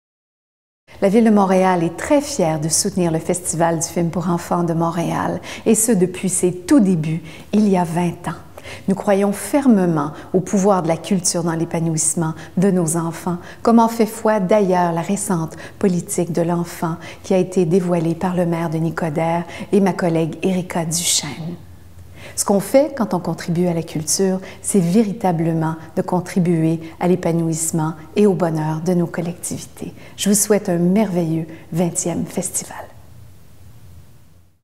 Mot de Manon Gauthier - FIFEM 2017
Mot de Manon Gauthier - Responsable de la culture, du patrimoine, du design, d'espace pour la vie et du statut de la femme - Ville de Montréal pour le FIFEM 2017

Merci à elle